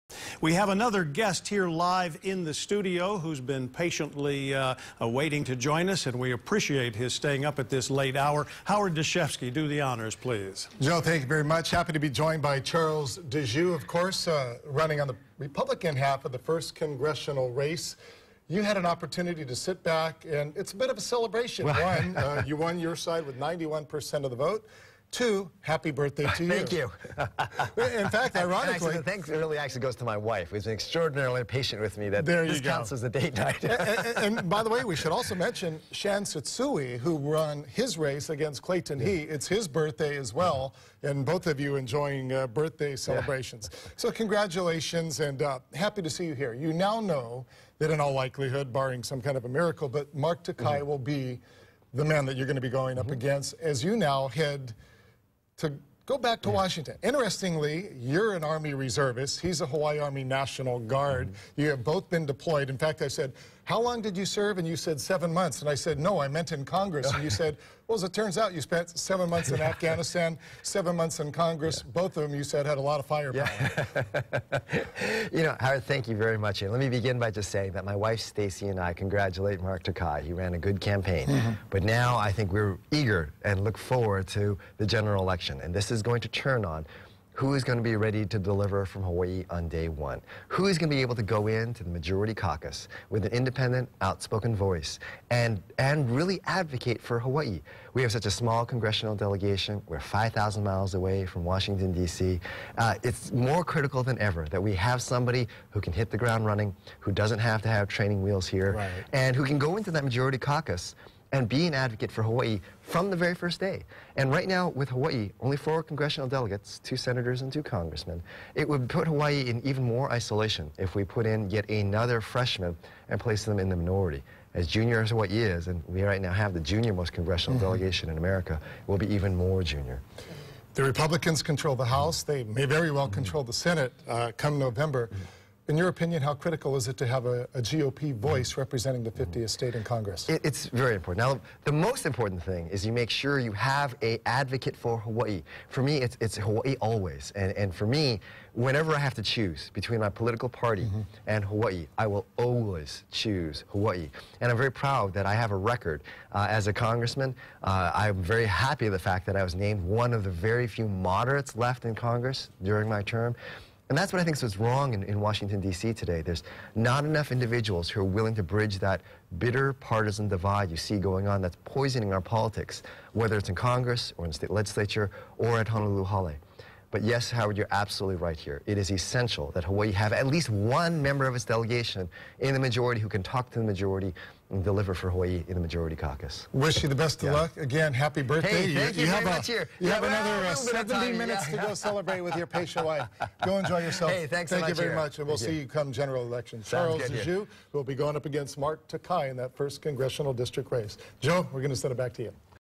Charles Djou discusses the election
Charles Djou sits down with KHON2's Howard Dashefsky to discuss the election.